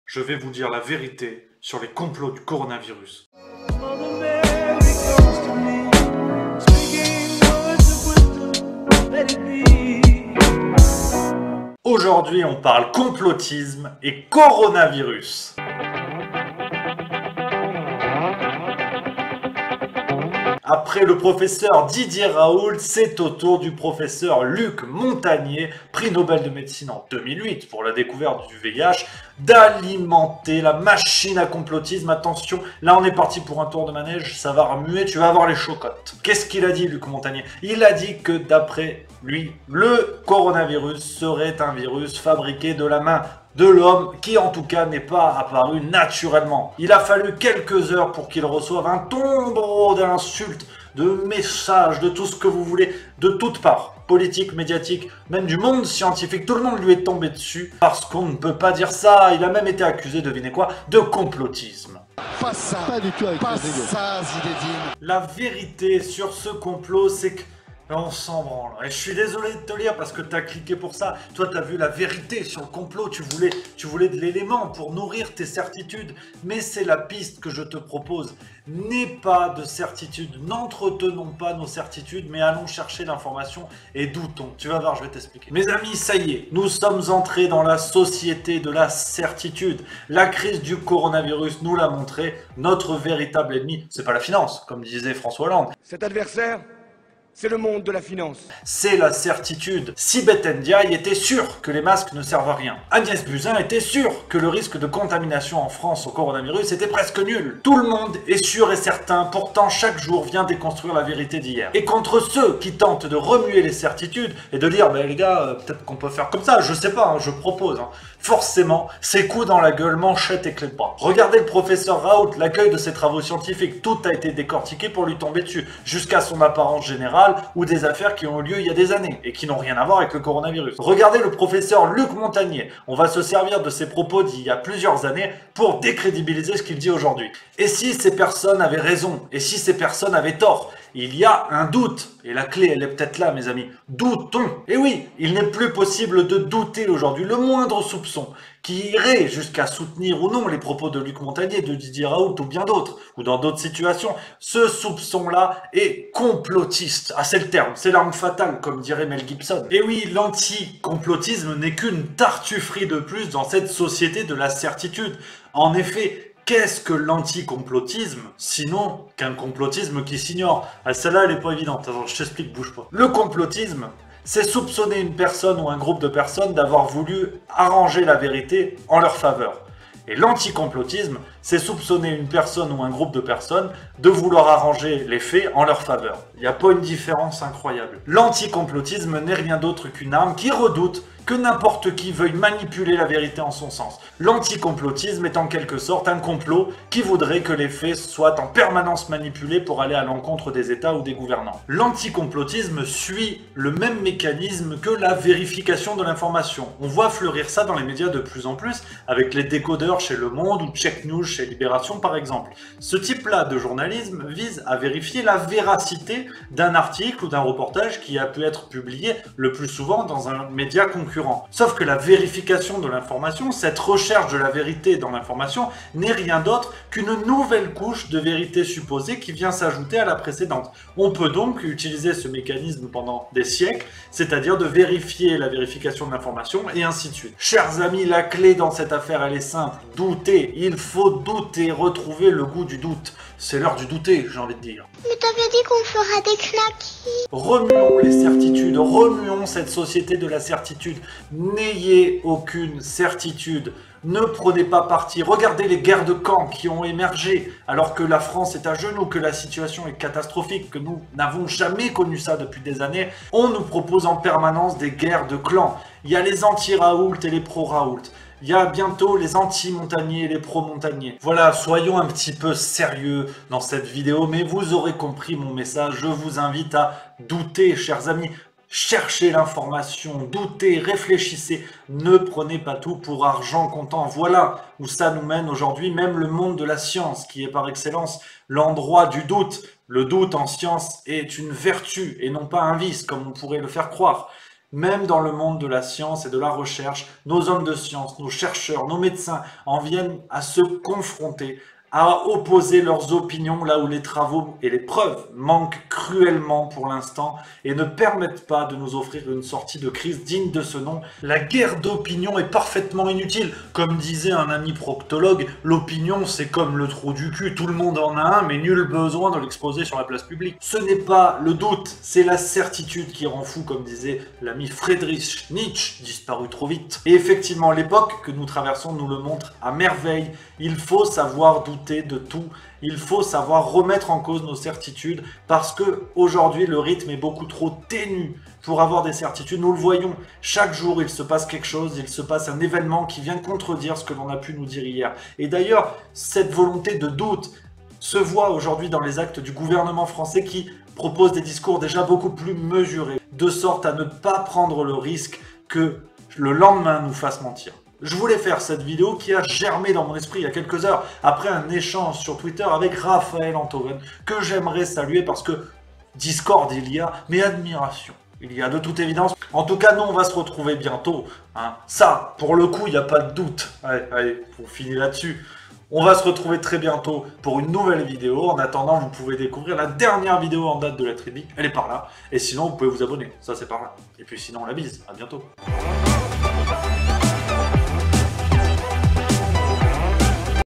Raoult, Montagnier, Buzyn : la VÉRITÉ sur les COMPLOTS ?
Après le scandale du Pr Didier Raoult, c'est le Pr Luc Montagnier qui fait parler de lui après avoir dénoncé la manipulation du coronavirus.

Lettres it be vous livre la vérité sur tous ces complots, sans aucun doute ?

Toutes les nouvelles chroniques et l'actualité des livres sont à retrouver